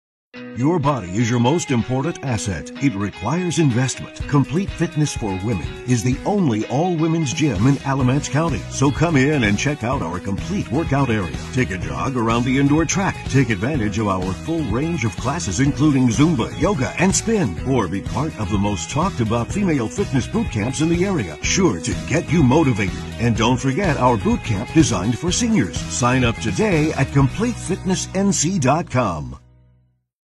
CFFW Video
Some of the many services that Complete Fitness for Women has to offer.